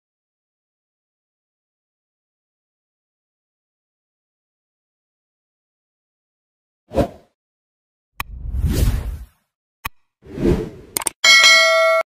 හුරුබුහුටි හා පැටියගේ වැඩ | a cute kit beautiful 🐇
පුංචි හා පැංචි තුවායක් මත සිටියි.

🔘 See more Videos ‎@skidpt